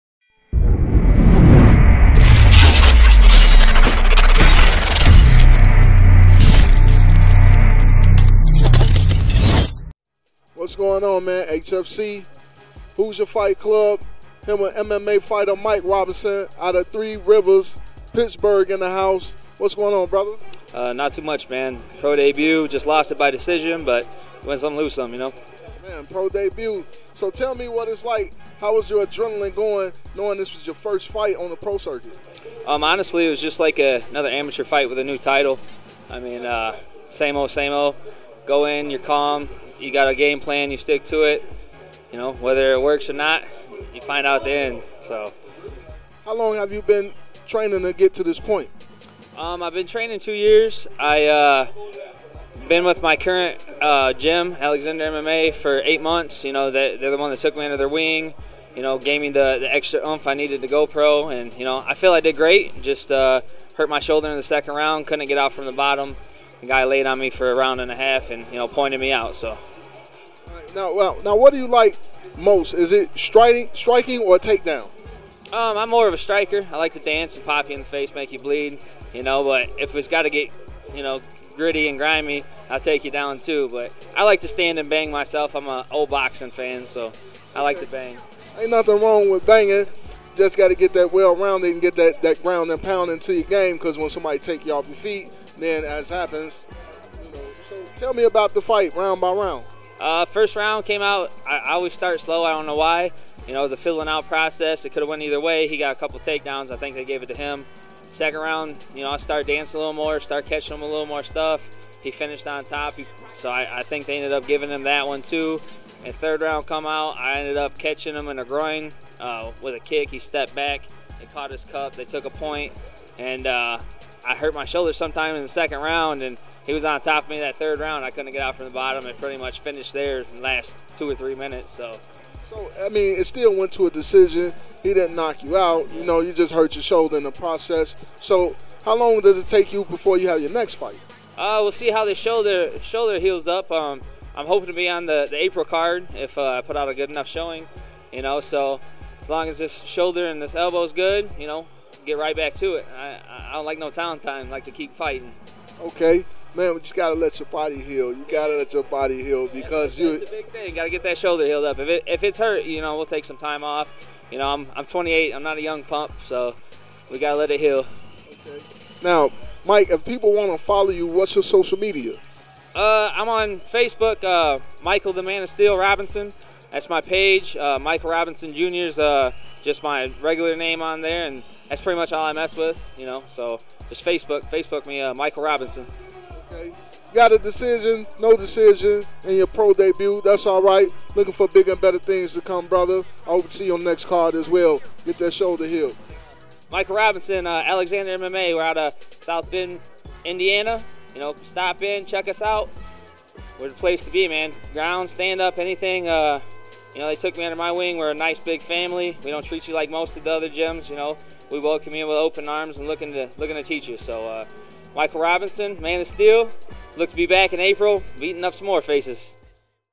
HFC27 Hoosier Fight Club Michael Man of Steel Robinson Jr
Hoosier Fight Club presents HFC 27 Live from the Blue Chip Casino
Urban Grind TV was on hand to catch all the action, here are some interviews and clips.
Chicago, Hip Hop Television Show, Comcast 25 Chicago Hip Hop Television Show, Rap, Media, Urban Grind TV, @UrbanGrindTV,